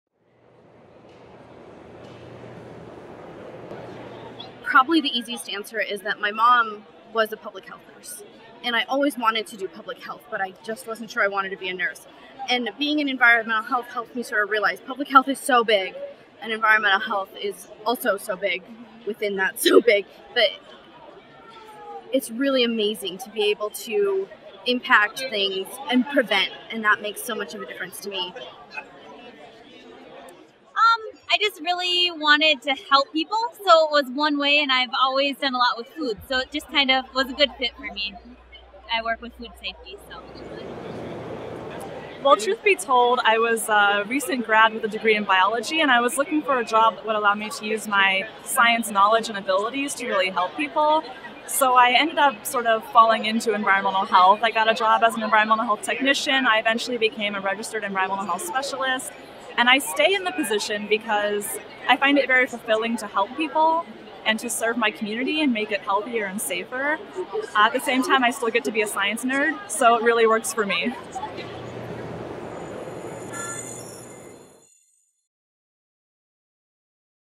NEHA Asks AEC 2016 Scholarship Winners: What motivated you to get into Environmental Health?
NEHA TV Asks "What motivated you to get into Environmental Health?" at the 2016 AEC.